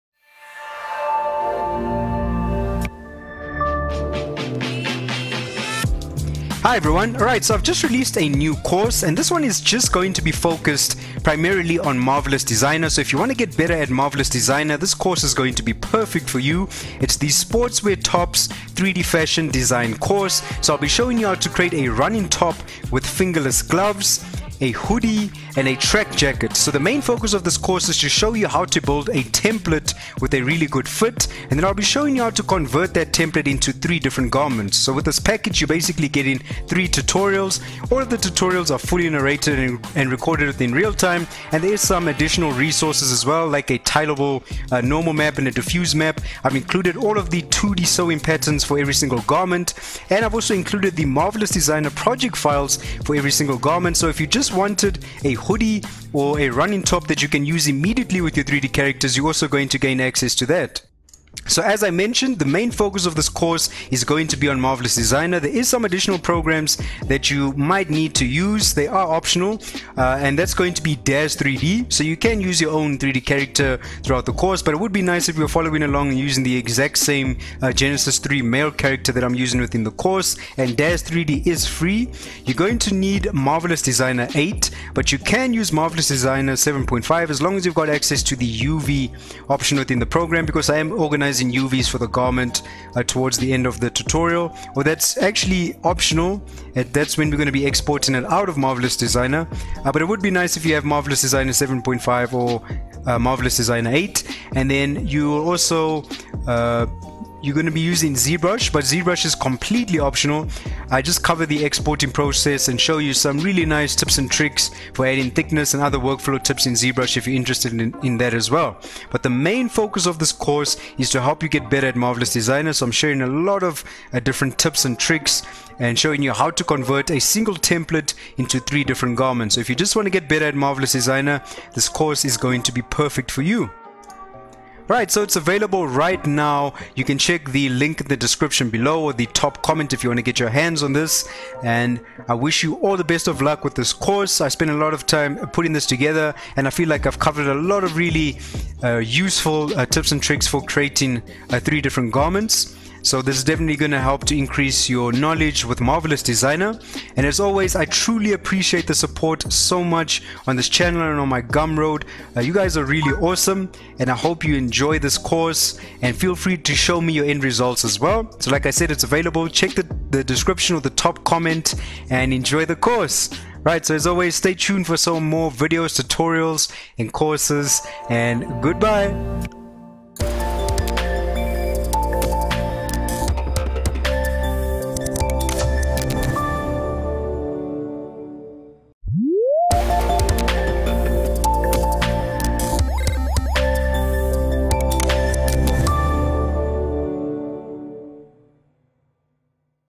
NEW COURSE - Sportswear Tops - 3D Fashion Design Course
Learn how to create a template garment in Marvelous Designer with a great fit and then convert that template into 3 different garments. If you're just looking at getting better at Marvelous Designer these tutorials will be perfect for you. Beginners are welcome to jump in and start learning. Enjoy!

What's Included?

- How To Create A Running Top With Fingerless Gloves

- How To Create A Hoodie

- How To Create A Track Jacket

- OBJ And Collada Files For Daz GEN 3 Male Character

- Zprj Project File For Each Marvelous Designer Garment (Completed Garments Ready To Use With Your 3D Characters)

- 2 Tileable Fabric Normal Maps + 1 Tileable Fabric Diffuse Map

- 2D Sewing Patterns For Each Garment

Difficulty

Level 1 (Beginner)

Level 2 (Intermediate)

Software Required

- Daz 3D (Not entirely required, you can use your own 3D models but ideally i would want you to follow along with the same character i'm using)

- Marvelous Designer 8 (If you have MD 7.5 that is fine, you just need access to Marvelous Designers UV Editor to complete the tutorials unless you know how to organize UV's outside of MD. I would highly recommend at least having MD 7.5 to complete this course)

- Zbrush 2019 (Completely optional but i decided to cover the export process and show you how to add thickness onto garments in zbrush)